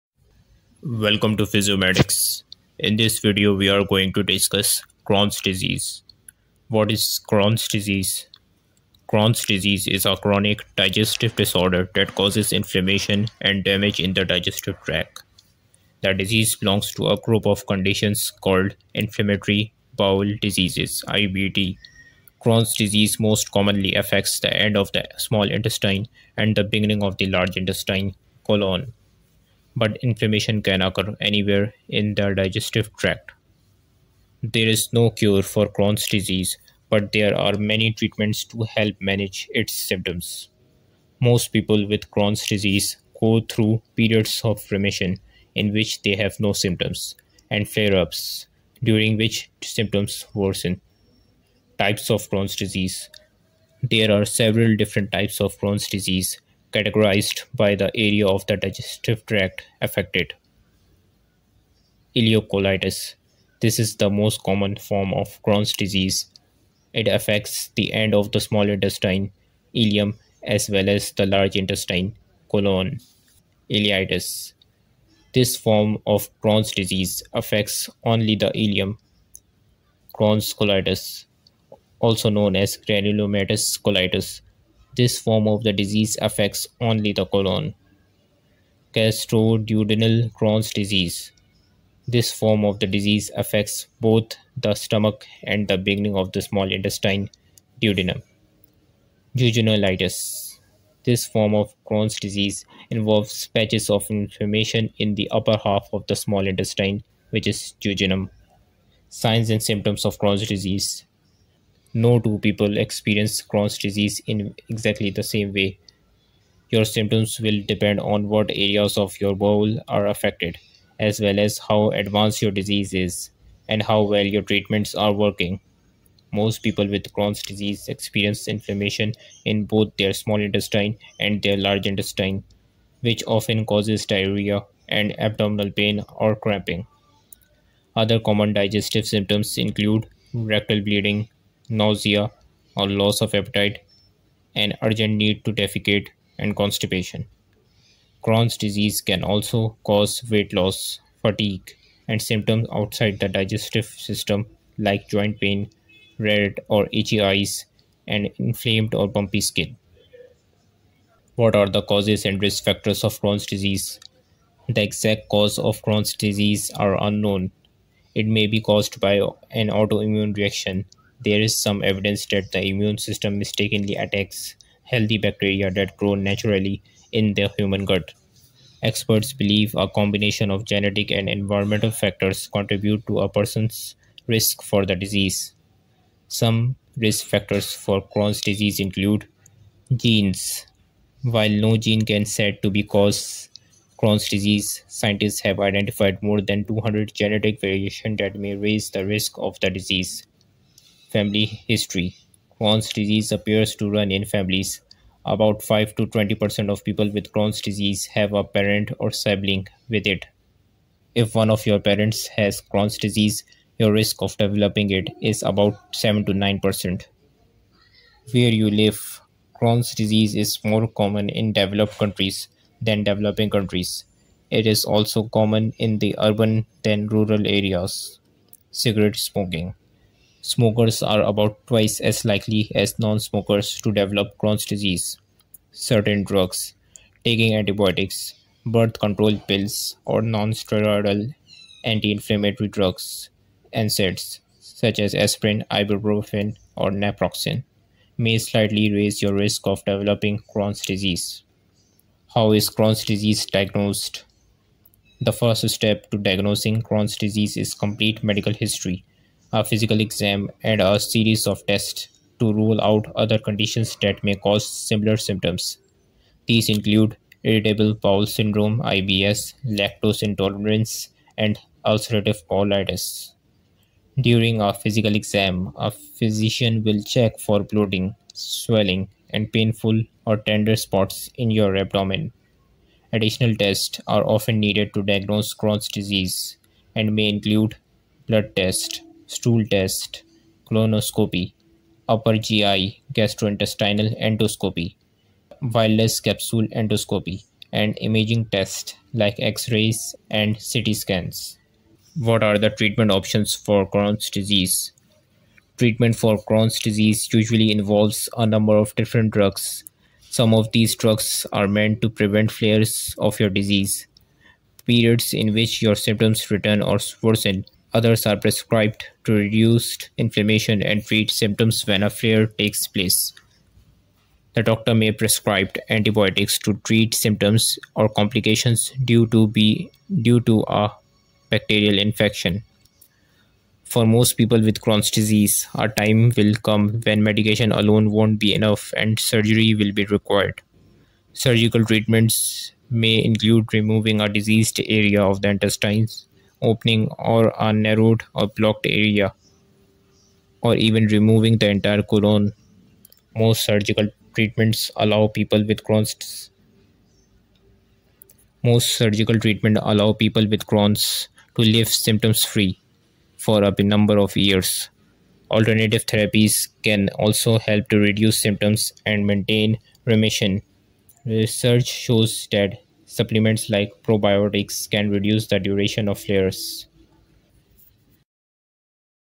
Crohns Disease Treatment - Crohns Disease Symptoms and Signs - Crohn's Disease Diet
Welcome to @PhysioMedics. In this video tutorial, we are going to discuss Chrons Disease treatment, its symptoms and signs.

Crohn's disease is a chronic digestive disorder that causes inflammation and damage in the digestive tract.

The disease belongs to a group of conditions called inflammatory bowel diseases (IBD).

Crohn's disease most commonly affects the end of the small intestine and the beginning of the large intestine (colon), but inflammation can occur anywhere in the digestive tract.

There's no cure for Crohn's disease, but there are many treatments to help manage its symptoms.

Most people with Crohn's disease go through periods of remission in which they have no symptoms, and flares during which symptoms worsen.

Crohn's Disease Symptoms and Signs:
No two people experience Crohn’s disease in exactly the same way.

Your symptoms will depend on what areas of your bowel are affected, as well as how advanced your disease is and how well your treatments are working.

Most people with Crohn’s disease experience inflammation in both their small intestine and their large intestine (colon), which often causes diarrhea and abdominal pain or cramping.

Other common digestive symptoms include rectal bleeding, nausea or loss of appetite, an urgent need to defecate, and constipation.

Crohn’s disease can also cause weight loss, fatigue, and symptoms outside the digestive system, like joint pain, red or itchy eyes, and inflamed or bumpy skin.

The crohn's disease pathophysiology is multifactorial and involves genetic predisposition, infectious, immunological, environmental, and dietary. The characteristic transmural inflammation can include the entire GI tract from mouth to the perianal area; although most frequently involve terminal ileum and right colon.

What causes crohn's disease?
The exact cause of Crohn's disease remains unknown. Previously, diet and stress were suspected, but now doctors know that these factors may aggravate, but don't cause, Crohn's disease. Several factors likely play a role in its development.

Immune system. It's possible that a virus or bacterium may trigger Crohn's disease; however, scientists have yet to identify such a trigger. When your immune system tries to fight off an invading microorganism or environmental triggers, an atypical immune response causes the immune system to attack the cells in the digestive tract, too.
Heredity. Crohn's disease is more common in people who have family members with the disease, so genes may play a role in making people more likely to have it. However, most people with Crohn's disease do not have a family history of the disease.

How does Crohn’s Disease Diagnosis is done?
The first step to diagnosing Crohn’s disease is complete medical history, a physical exam, and a series of tests to rule out other conditions that may cause similar symptoms. These include irritable bowel syndrome (IBS), lactose intolerance, and ulcerative colitis.

During a physical exam, a physician will check for bloating, swelling, and painful or tender spots in your abdomen.

Additional tests are often needed to diagnose Crohn’s disease and may include: blood tests, stool tests, colonoscopy, upper GI (gastrointestinal) endoscopy, wireless capsule endoscopy, and imaging tests like X-rays and computerized tomography (CT) scans.

There’s no single diet that helps all people with Crohn’s disease.

You may find that making certain dietary changes helps reduce uncomfortable symptoms during flares, such as bloating, gas, and diarrhea.

A good crohn's disease diet can be play a major role in preventing the disease and crohn's disease management.
Foods that commonly trigger symptoms include certain whole grains, nuts and seeds, raw fruits and vegetables, spicy foods, fatty or greasy foods, dairy products, and caffeinated or alcoholic beverages.

What is the difference between Crohn's and colitis?
Both conditions affect the bowel, but in slightly different ways. Crohn's disease causes inflammation of the full thickness of the bowel wall, in any part of the digestive tract from the mouth to the anus. Ulcerative colitis is inflammation of the inner lining of the large bowel (colon and rectum).